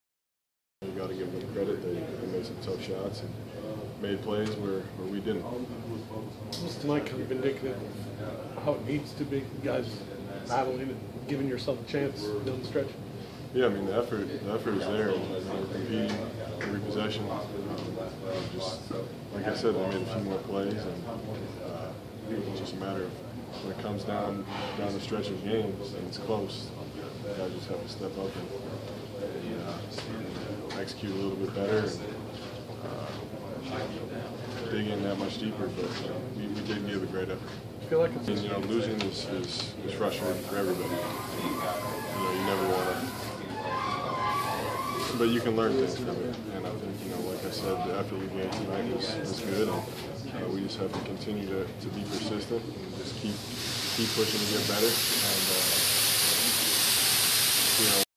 Jon Leuer
Griz forward Jon Leuer explains loss to Lakers